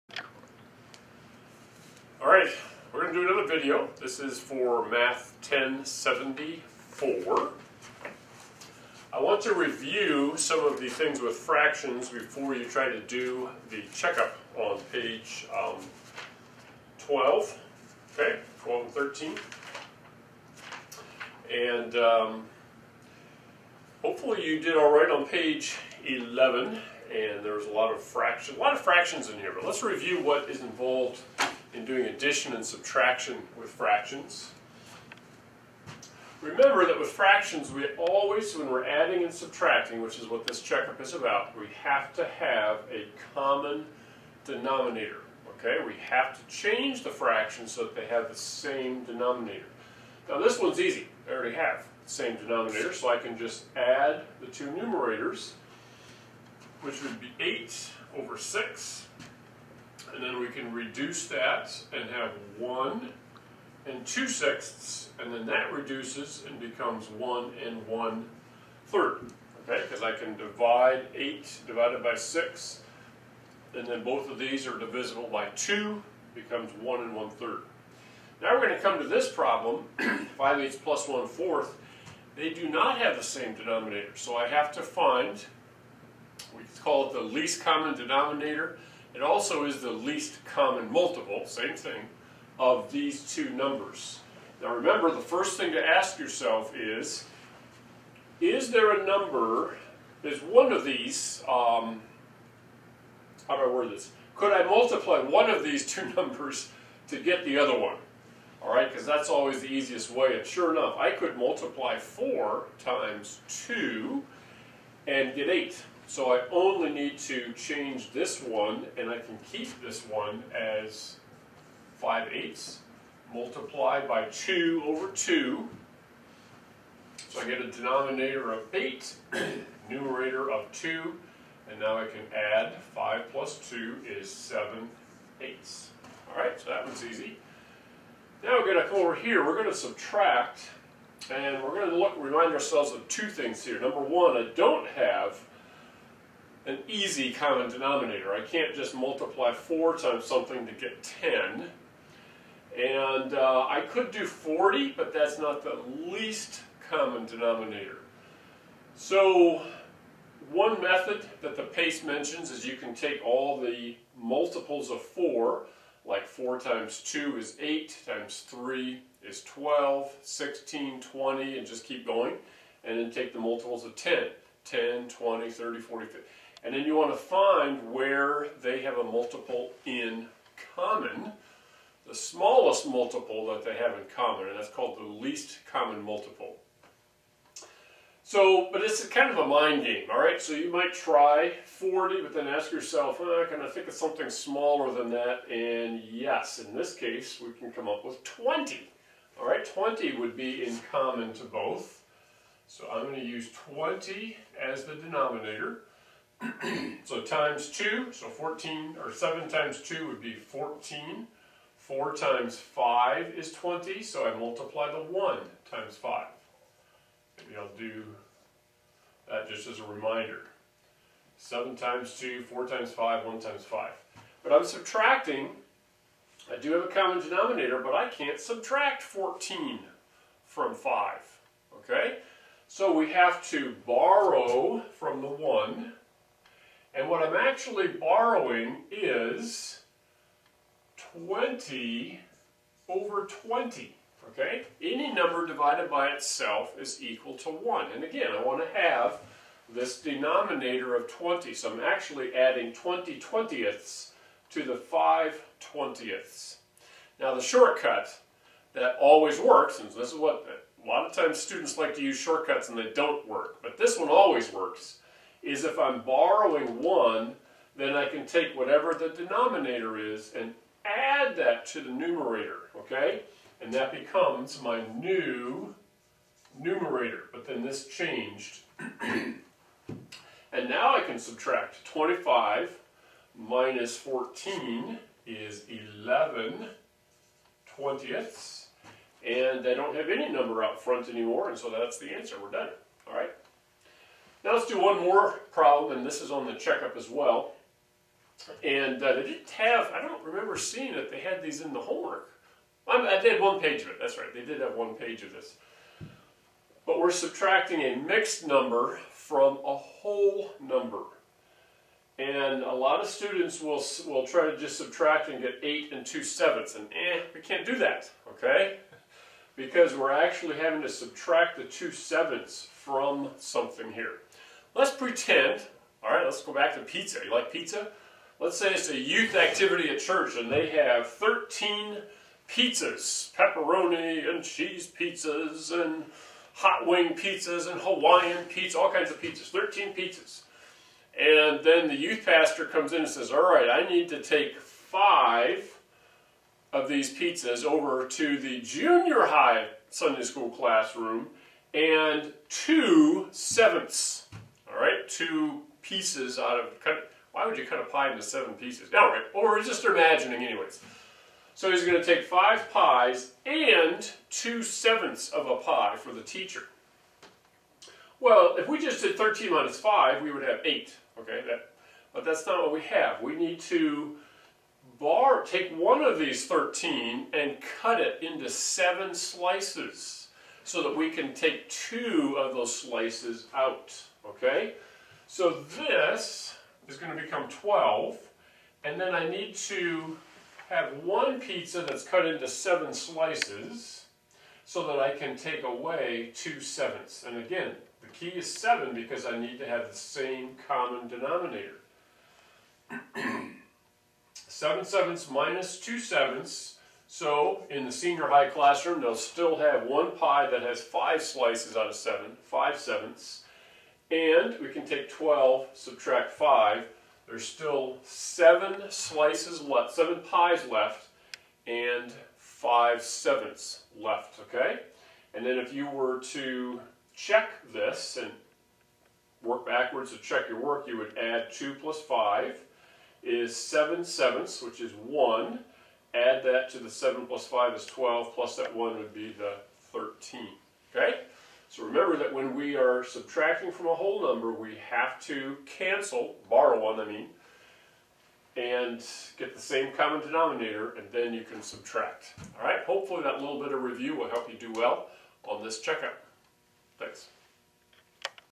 Math 1074 - p. 12-13
This video is about Math 1074, just before the Check-Up on pp 12-13. We review the rules for adding and subtracting fractions and mixed numbers.